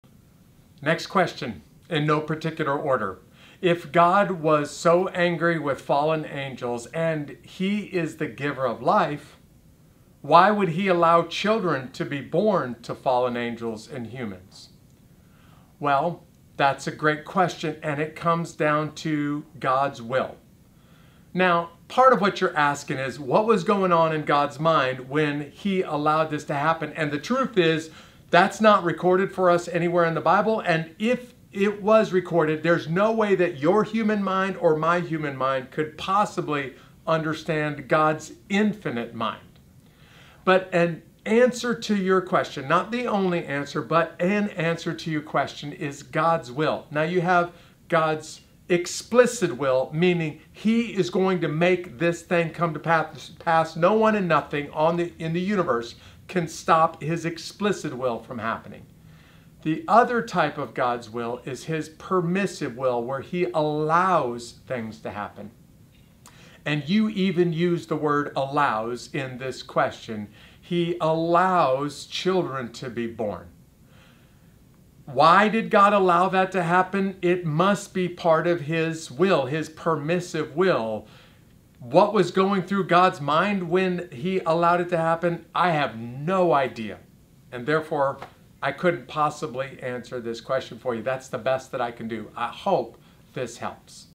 Why did God allow the Nephilim?
If God was so angry with the fallen angels and he is the giver of life, why would he allow children to be born to fallen angels and humans?